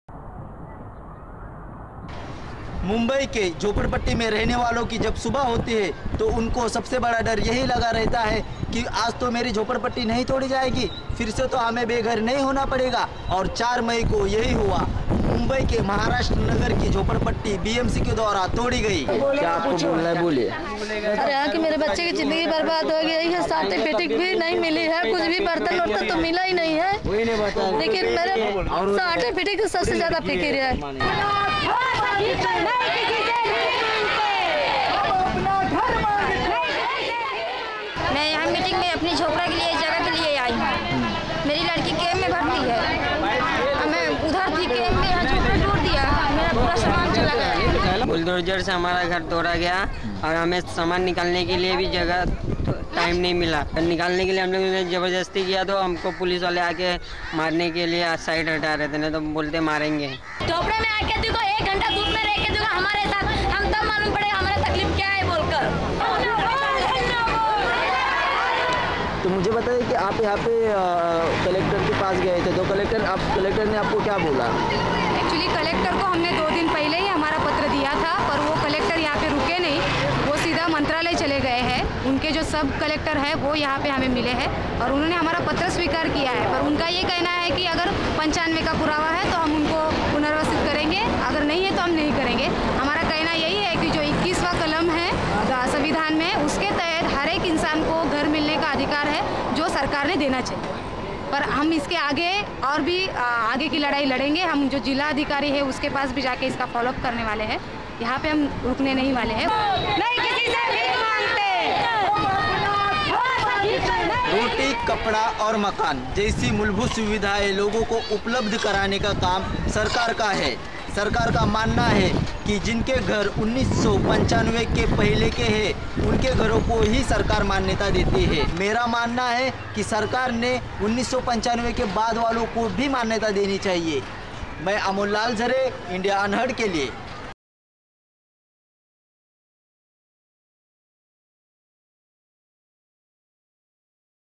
Fighting For a Home in Mumbai Slums
In Mumbai, slum dwellers are unremittingly struggling to secure housing. In a city like Mumbai, whose size has tripled since India's Independence, and has surpassed 20 million inhabitants -- assuming that any census performed in such a rapidly expanding metropolis can be reliable -- space constitutes the greatest wealth. For a multitude, intimacy is a distant dream, obtained at the price of a cinema ticket that grants its holder some hours of privacy and tranquility in the dark room of the cinema hall. Yet, in spite of the tremendous human density, pollution, discomfort, lack of infrastructure and the accompanying violence, migrants keep flowing into the city, in search of jobs and better life. They swell the ranks of slum dwellers, who already make up over 60% of Mumbai's population. Slums today constitute an integral part of the city, providing a precarious solution to the extreme spatial challenge of the biggest Indian metropolis. But because space is scarce and precious, slum residents face the attacks of developers as well as the Mumbai Municipal Corporation. Developers intend to raze the slums and make profit out of the vacated land. Worse, they receive the support of a government that envisions a slum-free city. On paper, the slum rehabilitation policy provides the Mumbai Municipal Corporation with a legal framework to destroy slums built after 1995 and force the relocation of its residents. This controversial policy allows developers to grab any land in exchange for building free houses for slum dwellers. But only those able to prove that they have been residing in the slum prior to 1995,- in rare cases before 2000 -- are entitled to the scheme. As a result, 70% of slum dwellers in the city are ineligible for these benefits, and stand to lose everything if their homes are razed. With this in mind, it is easy to understand the stiff opposition of Mumbai slum dwellers. Even those entitled to free housing often oppose the policy, on the ground that relocation means moving away from their community, from a network of mutual aid and support, and often from sources of employment. It is only with these elements in mind that one can understand the anger and distress of the 3000 Maharashtra Nagar slum residents. On March 4th, 2011, their slum was completely demolished by the Municipal Corporation. Many of them watched as their lives scattered that day; losing precious documents, utensils and all other belongings. Amid the turmoil, the residents did not lose hope and mobilized to reclaim their rights over the land they had occupied so far. Most of the slum dwellers have been living there for years, but proving their rights over land might turn out to be impossible, given that most of them do not possess any documents attesting their rights or that these documents have been lost or destroyed when the slum was razed. What's more, many of them settled after 1995, and as a consequence, are not entitled to any rehabilitation. In any case, what most of the residents want is merely to obtain decent housing within the slums they used to live in. The slum provided them with a community, with a sense of belonging that they cannot find elsewhere in the city. What they expect from the government is merely to make this place inhabitable. As it is often the case in Mumbai, the cases linger on for weeks, or even months, while slums are reconstructed, after only hours or days, because the dwellers have nowhere else to go in the inhospitable metropolis. In the fight over space, slum dwellers might be weak and disarmed when facing the developers and bulldozers, but they are numerous and unremitting, and above all, they have nothing to lose.